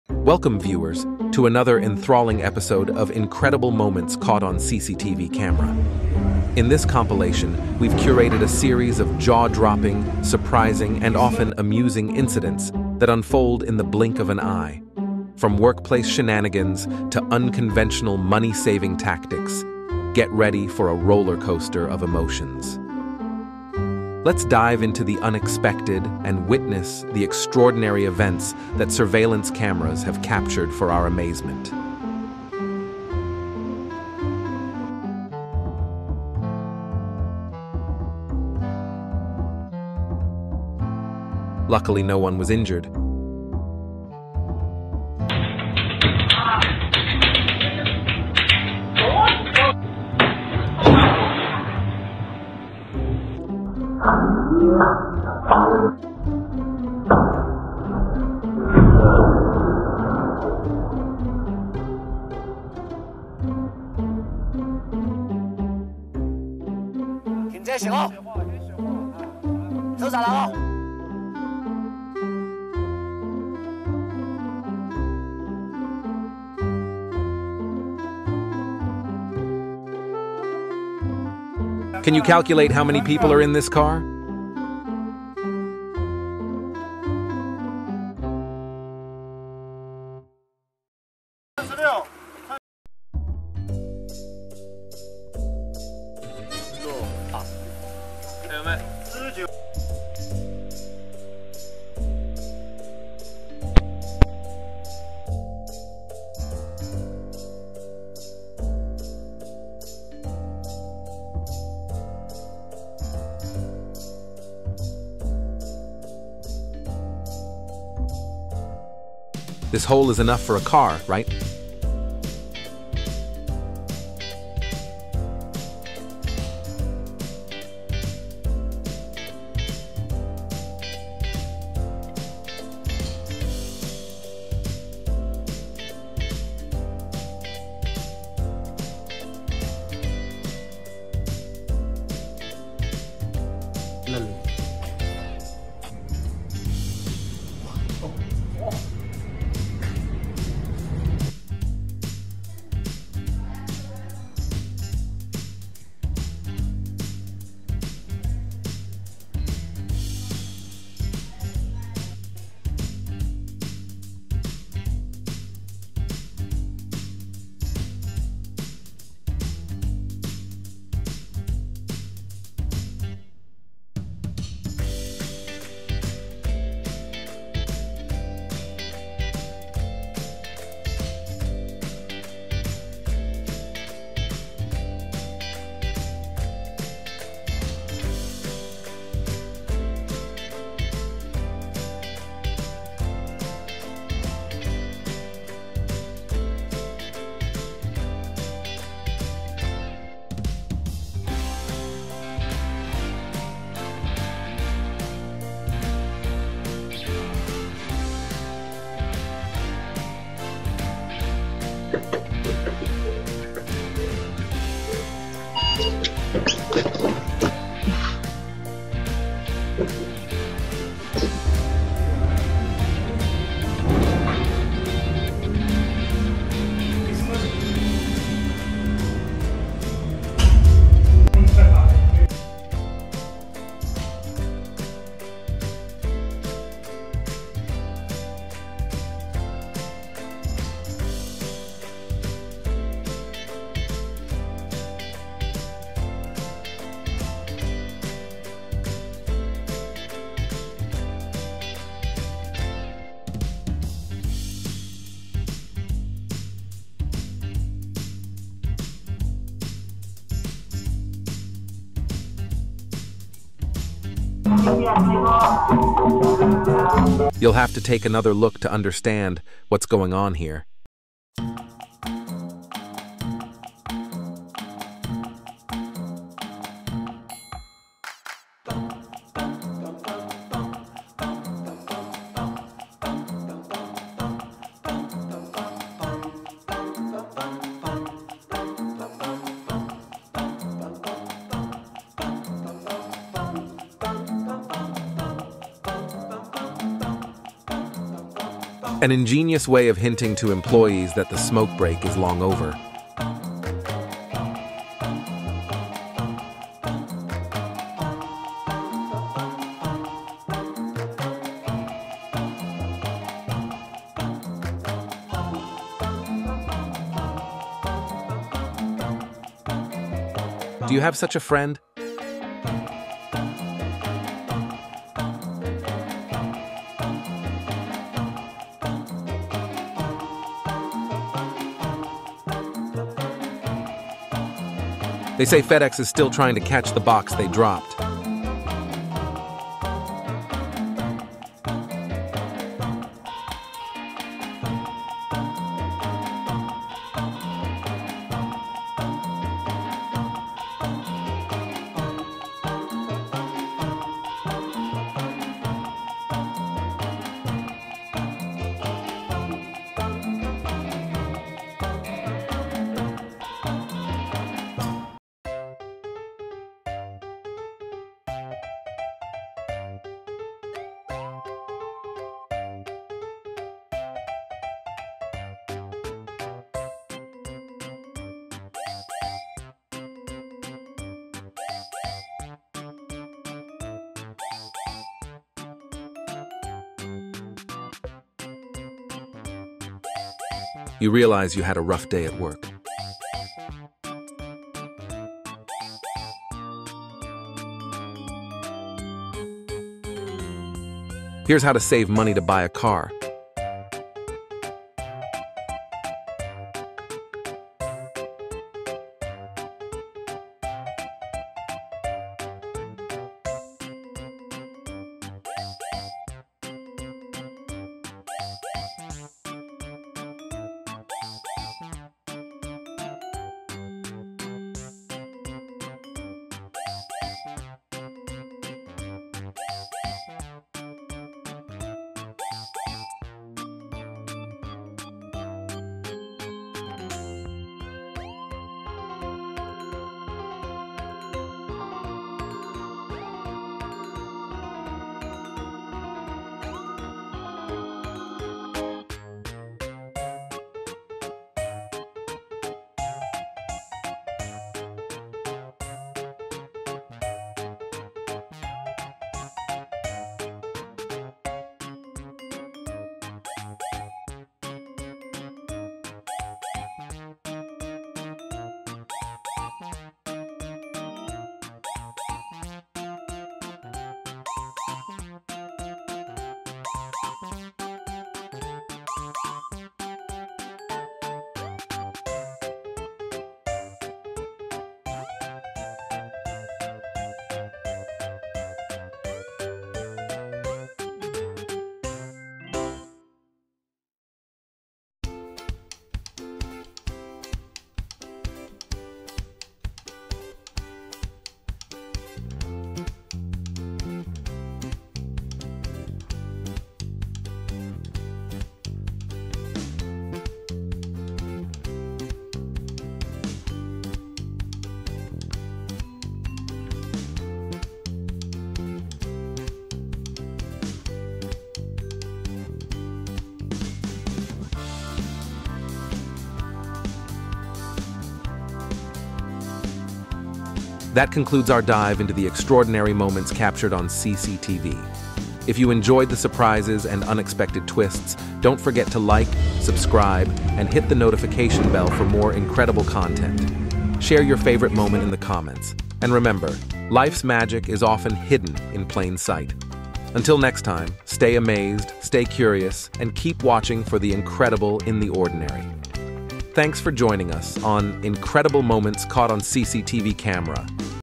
70 Incredible Moments Caught on CCTV Camera
70 Incredible Moments Caught on CCTV Camera Welcome, viewers, to another enthralling episode of "Incredible Moments Caught on CCTV Camera"! In this compilation, we've curated a series of jaw-dropping, surprising, and often amusing incidents that unfold in the blink of an eye. From workplace shenanigans to unconventional money-saving tactics, get ready for a rollercoaster of emotions. Let's dive into the unexpected and witness the extraordinary events that surveillance cameras have captured for our amazement.